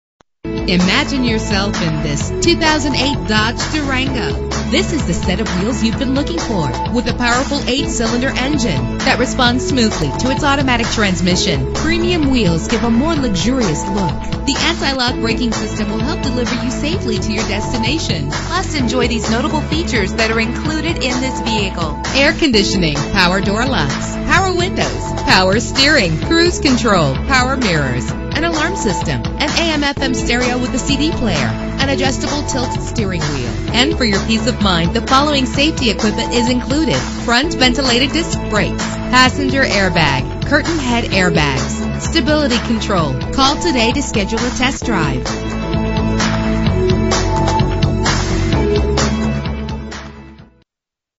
2008 Dodge Durango - LAS VEGAS NV
Year: 2008
Make: Dodge
Model: Durango
Trim: SXT
Engine: 4.7 liter 8 cylinder 16 valve
Transmission: Automatic
Color: Blue
Mileage: 25859
Address:
6800 S. TORREY PINES